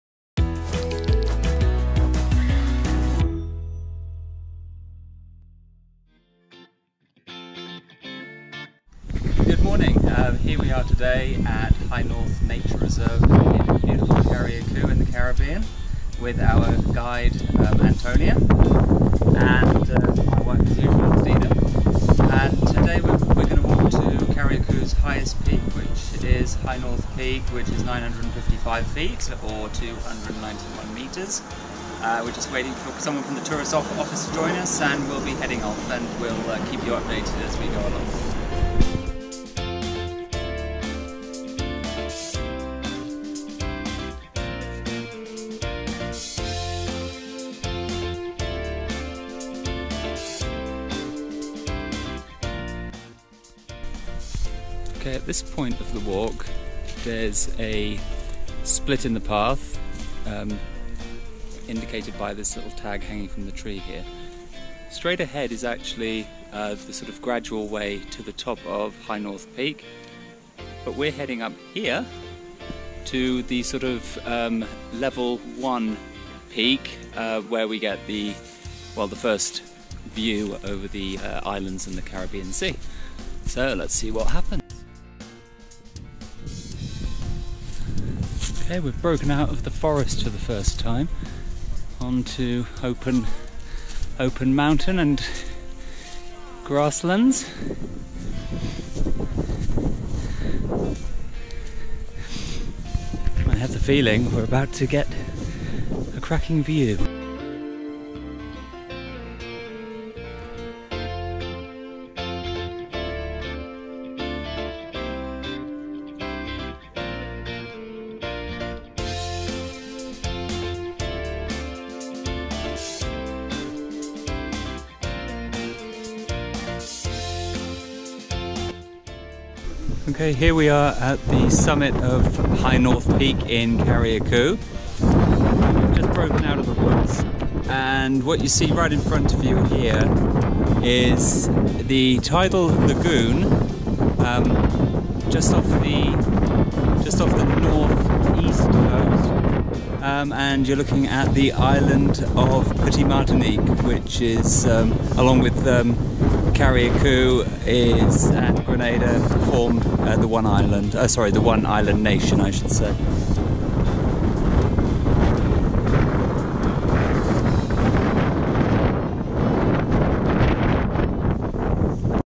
High North Peak Hike in Carriacou, Caribbean
Starting Point for the High North Peak Hike

There was no indication as to where the actual trail commenced, just a small opening in the undergrowth off to our right. In fact, the main reason why Dario and his team were joining us was to plan upgrades to the trail to make it a more user-friendly.

The High North Trail

The initial section of the High North Peak hike is moderately steep and takes you along a trail that is not very well defined. For the most part you could easily think that there was no dedicated trail at all.

The track meanders through the fairly dense dry deciduous forest consisting of a variety of trees, plants and shrubs. Owing to the thickness of the vegetation, there are limited views to be had and therefore no external reference points to assist with your general sense of direction.

Within a short distance, there is a path that heads off to the left, marked by a red cable tie on an overhanging branch. We only knew that this was the indicator for this turn thanks to members of our hiking group.

You will head up this path for a couple of minutes and as you do the canopy that has been shielding you opens and the Caribbean sky greets you once more. You now find thick wild grass underfoot as you make your way to the first of three vantage points from where to soak up the views on offer.

Vantage Point 1

Here you get to enjoy sweeping views right down the west coast of Carriacou from the main town of Hillsborough along to Paradise Beach. You will clearly see the island’s second-highest peak of Chapeau Carre as well as the three small islands that sit a short distance off the west coast: Jack-A-Dan, Sandy Island and Mabouya.

Once you have enjoyed the wonderful views on offer, return to the main trail and turn left. This next stretch of the hike is fairly flat. Off to your right, you will now spot the remnants of a 2 metre (6 foot) high stone wall which during colonial times was used as a cattle pen. It apparently dates back to around 1750.

Shortly after, a number of Century plants will be visible off to your right along with one of the few permanent signs that indicate that you are on the right track for High North Peak.

The trail now steepens as you make your way towards the plateau of the national park. This section of the hike once again leads you through thick, wild forest. It is only once you reach the top and vantage point 2 that you once again get to feast your eyes on the wonderful views.

Vantage Point 2

From this location, you now get to look south along the green forest-clad interior of Carriacou. Better still, are the views you now have out to the east. The beautiful, vibrant turquoise waters of the tidal lagoon sit just off the coast, beyond which are the smaller, islands of Petite Martinique and Petit Saint Vincent (right to left).

Just a stone’s throw to the south is the third and final location to admire these enticing Caribbean vistas.

Vantage Point 3

Once again, you get to enjoy the spectacular southerly views right down the length of Carriacou. But you can now take in these views whilst seated on a couple of large rocks that are literally perched right off the southern tip of the summit plateau.

From this point the High North Peak hike loops in a clockwise direction hugging the outer edge of the summit plateau.

There was really no discernible track for this stage of the hike. It was simply a case of picking our way between trees and through undergrowth. Thankfully, Hansel came equipped with a cutlass so he lead the way clearing the path as he went.

Dario kept emphasising that we all need to be more aware of our environment and always be on the lookout for the array of wonderful things that surround us. Almost immediately, Sarah gazed up into the canopy high above and spotted an Iguana happily basking in the sunshine.

Shortly afterwards, Dario pointed out the tiny nest of a Hummingbird. What a beautifully intricate work of art that these little creatures are able to create.

Being a part of this rich and very diverse environment, so far removed from our day to day reality, helped make our experience even more memorable.

Giraffe Tree

As we headed north along the western summit ridge, Dario pointed out a tree that resembled a giraffe. I am not kidding you, it really does look like a giraffe! It is just amazing what nature is capable of, even if it is totally by accident.

Eventually, the loop trail rejoined the path on which we ascended High North Peak. Here we turned left and followed the track back to the trailhead.